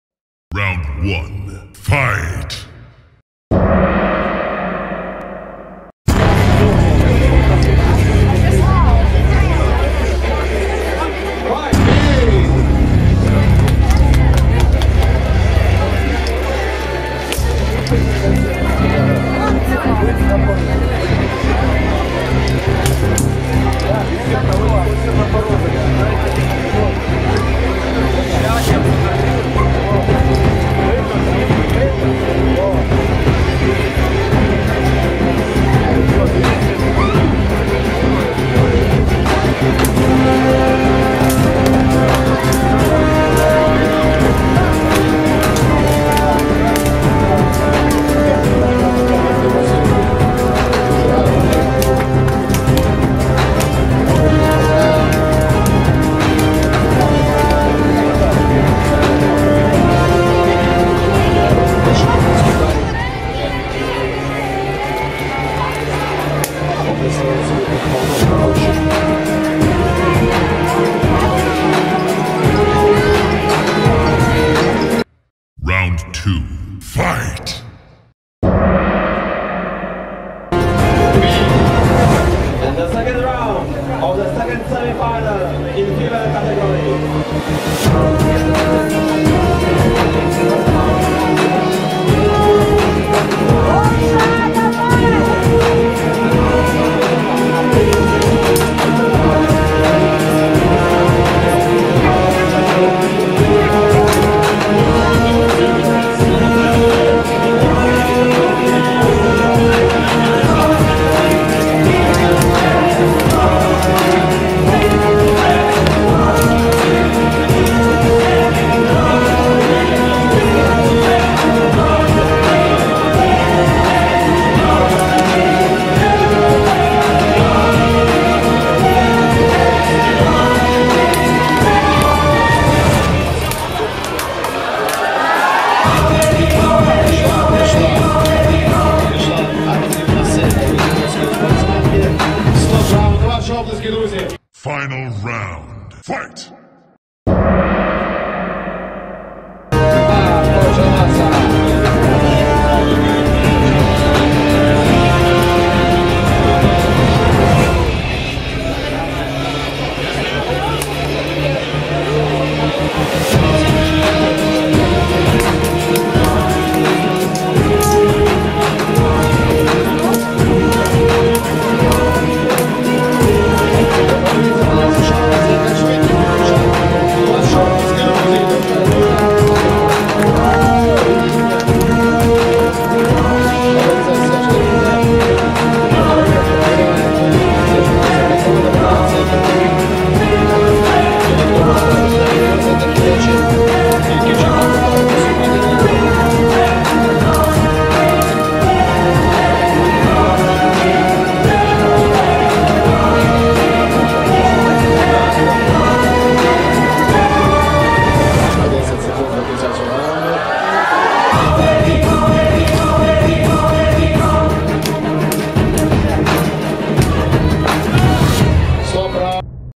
Shield and Sword - Medieval Combat - Poland vs Moldova - semi final [Female]
Poland vs Moldova - semi final [Female]